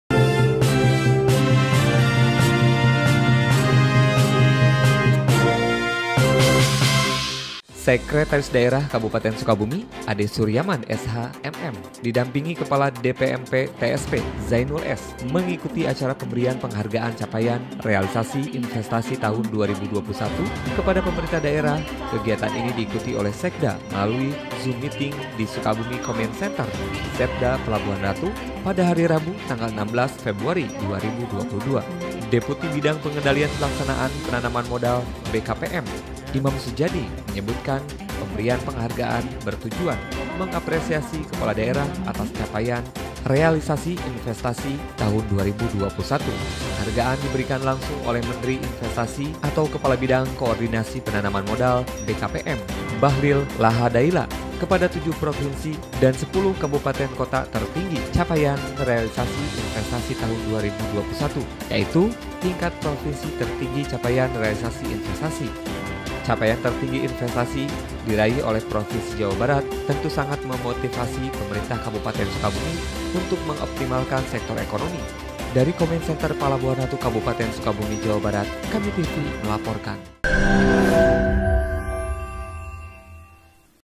JABAR TERTINGGI REALISASI INVESTASI 2021
Sekretaris Daerah Kabupaten Sukabumi  Ade Suryaman, SH., MM didampingi Kepala DPMPTSP Zaenul, S mengikuti acara Pemberian Penghargaan Capaian Realisasi Investasi Tahun 2021 kepada Pemerintah Daerah. Kegiatan ini diikuti oleh Sekda
melalui Zoom Meeting di Sukabumi Command Center Setda Palabuhanratu. Rabu (16/2/2022)

Deputi Bidang Pengendalian Pelaksanaan Penanaman Modal BKPM, Imam Soejoedi
menyebutkan pemberian penghargaan bertujuan mengapresiasi kepala daerah atas capaian Realisasi Investasi Tahun 2021

"Pemberian penghargaan  Untuk memberikan apresiasi kepada kepala daerah atas capaian realisasi investasi sebesar 901 trilyun apalagi di masa pandemi covid tentunya koloborasi  dengan pemerintah daerah juga rekan rekan DPMPTSP sangat menunjang kinerja capaian realisasi investasi ini"Jelasnya

Penghargaan diberikan langsung oleh Menteri Investasi/Kepala Badan Koordinasi Penanaman Modal (BKPM) Bahlil Lahadalia kepada 7 provinsi dan 10 kab/kota tertinggi capaian Realisasi Investasi Tahun 2021, Yaitu:

Tingkat provinsi tertinggi capaian realisasi investasi
1. Provinsi Jawa Barat
2. Provinsi DKI Jakarta
3. Provinsi Jawa Timur
4. Provinsi Banten
5. Provinsi Riau
6. Provinsi Jawa Tengah
7. Provinsi Maluku Utara

-Tingkat Kab/kota
1. Kabupaten Bekasi
2. Kota Surabaya
3. Kabupaten Halmahera Utara
4. Kabupaten Morowali
5. Kabupaten Karawang
6. Kabupaten Mimika
7. Kabupaten Konawe
8. Kota Balik Papan
9. Kota Cilegon
10. Kabupaten Gresik

" Alhamdulilah Jawa Barat peringkat Pertama raihan tertinggi capaian Realisasi Investasi Tahun 2021, selamat untuk Jawa Barat serta tidak lupa juga saya sampaikan selamat kepada Kabupaten Bekasi dan Karawang yang sudah mendapatkan penghargaan" Ungkap Sekda usai mengikuti acara

Menurutnya, capaian tertinggi investasi yang diraih oleh Provinsi Jawa Barat tentu sangat memotivasi  Pemerintah Kabupaten Sukabumi untuk mengoptimalkan sektor ekonomi.

" Kita termotivasi dan akan mendorong perkembangan ekonomi di Kabupaten Sukabumi " pungkasnya